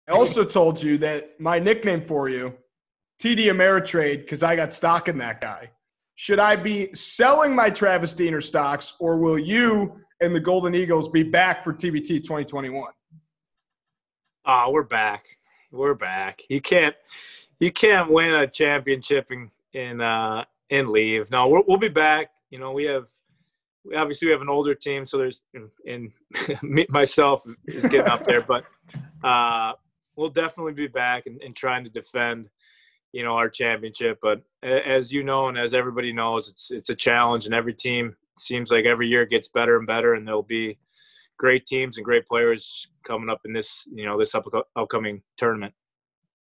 Travis Diener announces the Golden Eagles will return for TBT 2021!
TBT features 64 teams of top professional, college alumni, and international basketball players competing in a single-elimination 5-on-5 tournament for a winner-take-all $1 Million prize.